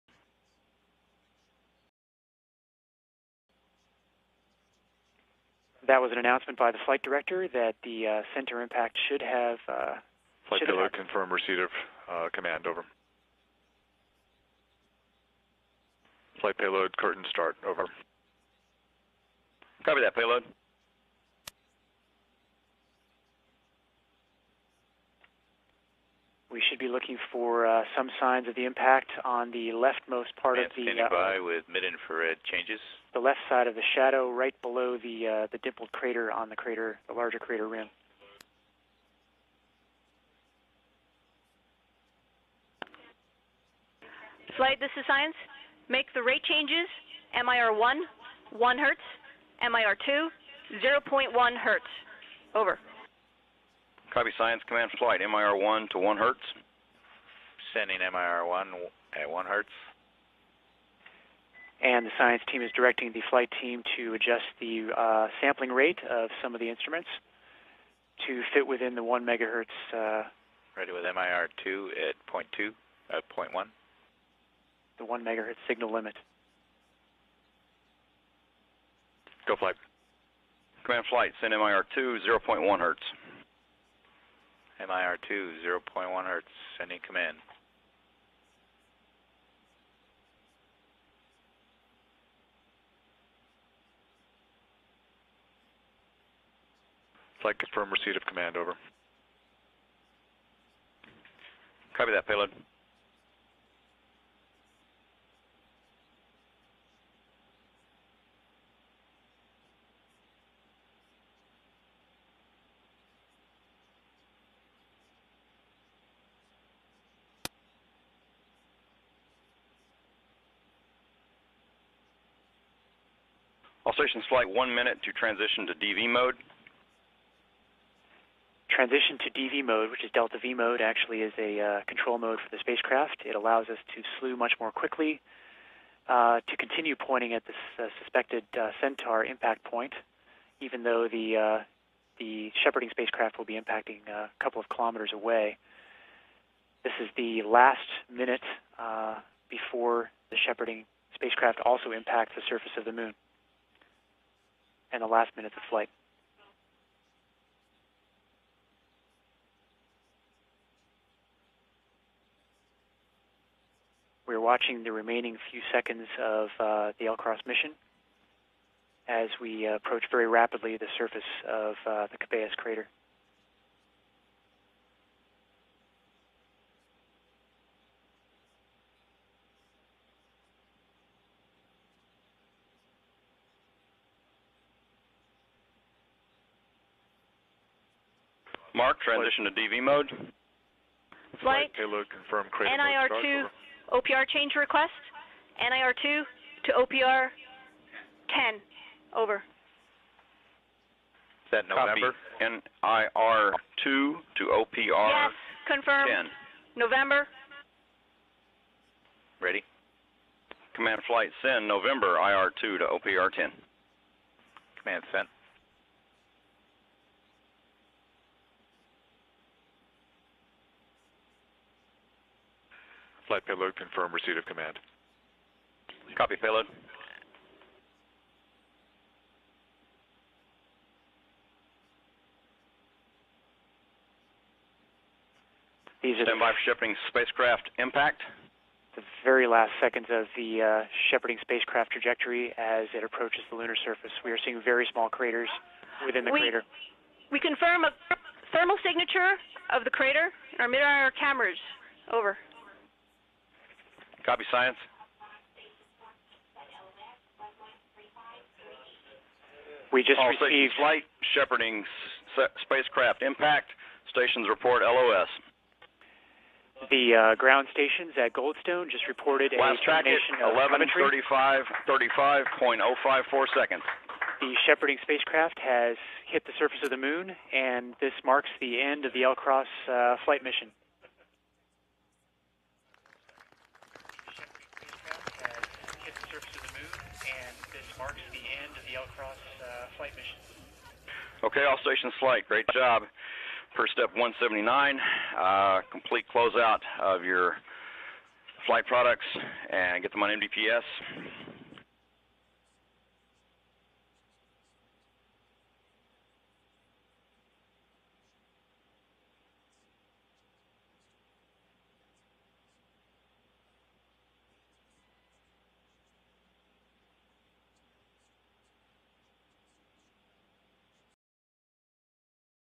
LCROSS: IMPACT
October 9th, 2009- This is the video showing the impact of the Lunar CRater Observation and Sensing Satellite (LCROSS) to the lunar surface (both Centaur and the shepherding spacecraft)